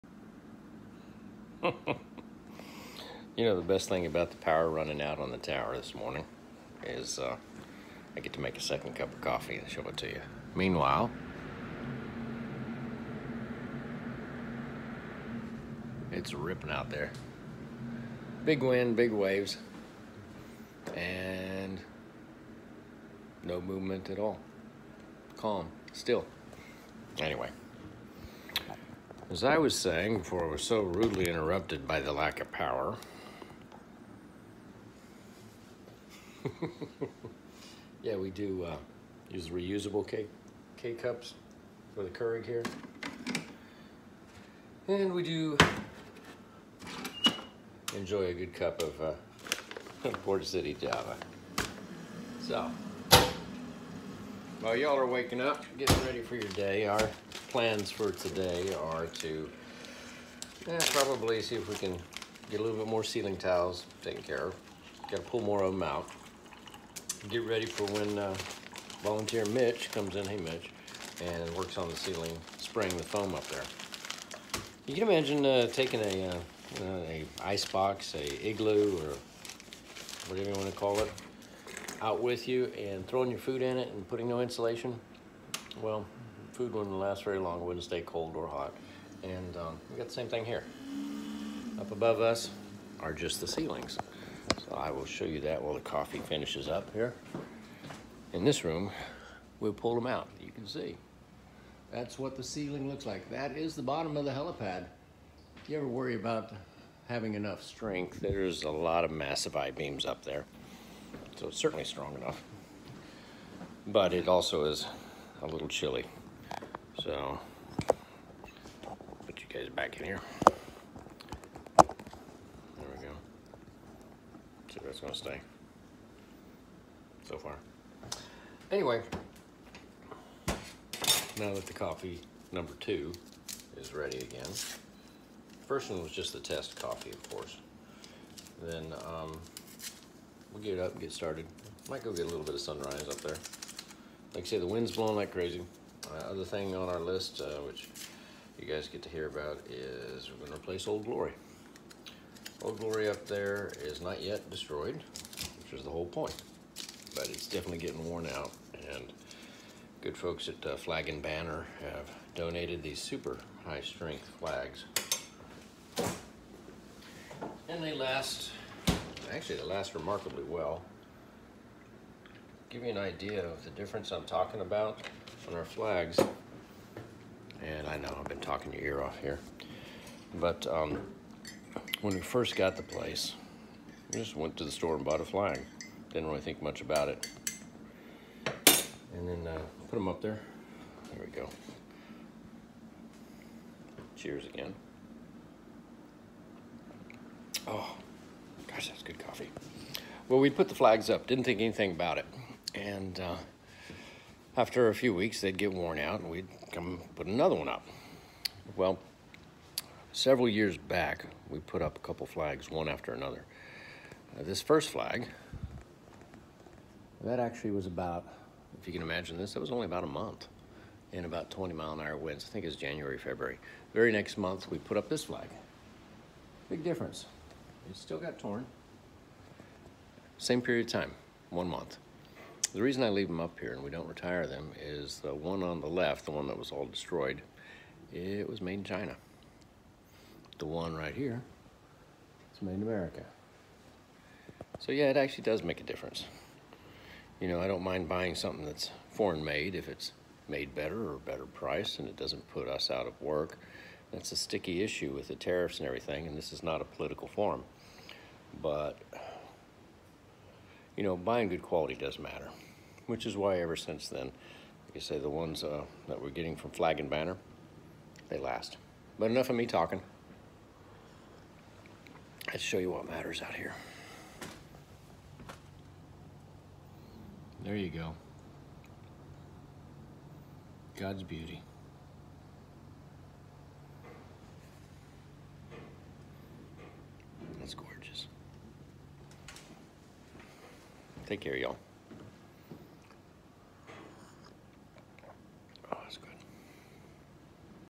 Coffee take 2... Made in America
2nd cup of port city Java after refueling the generator and a brief walk about…